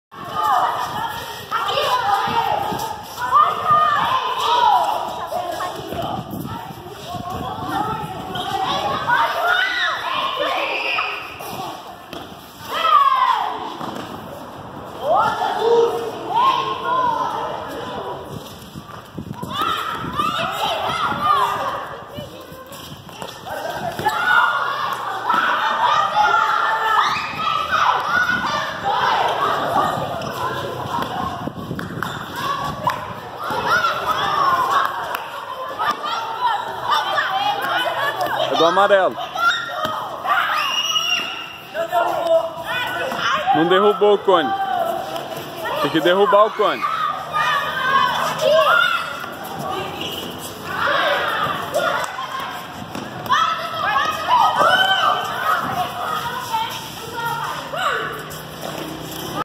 Queimada Boliche 🎳🎳🎳 (326) Educação Física Escolar - Acylino Amaral Gurgel - Salto-SP
Queimada Boliche 🎳🎳🎳 Introdução aos princípios defensivos e ofensivos dos esportes coletivos. Desenvolvimento de pensamento estratégico. (6ºAno D)